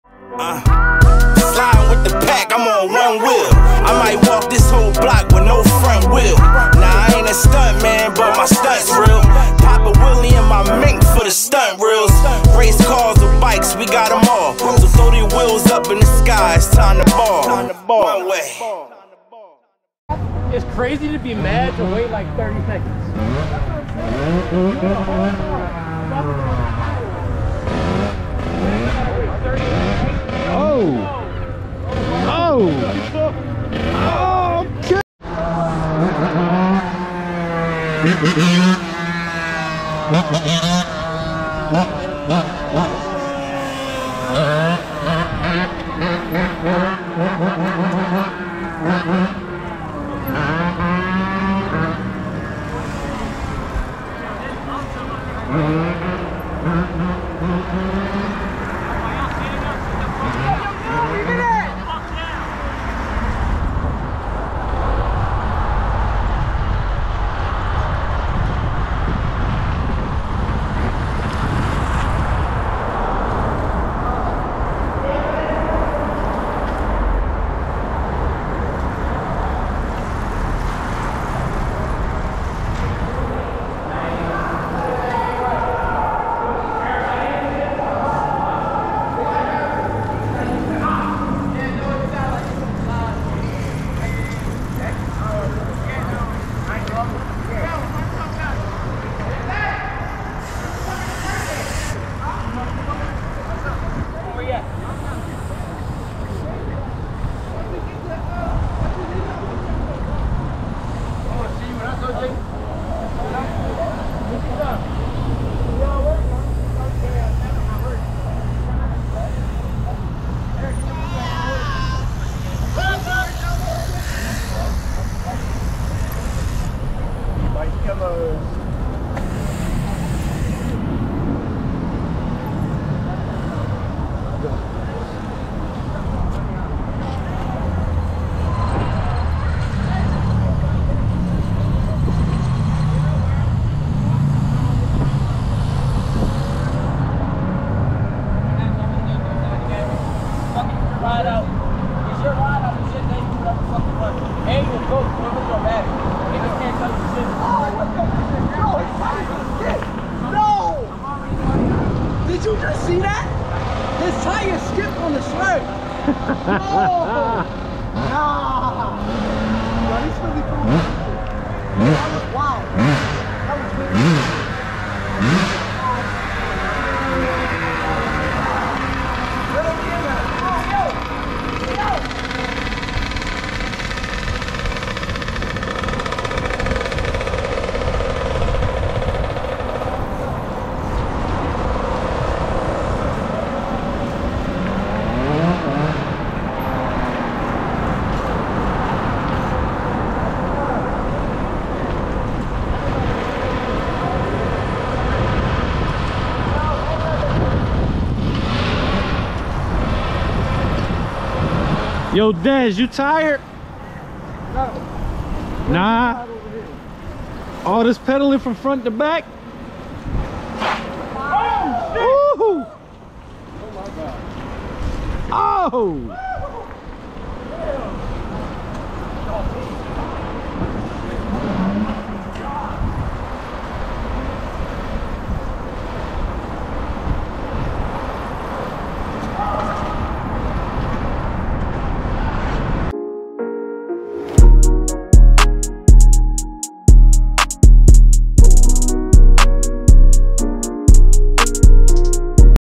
One Wheel Wheelie Battle
This was a new years day 2023 ride out. Weather was nice.   pulled up to philly from nyc and  showed up from Baltimore and we got the footage of these two on one wheel.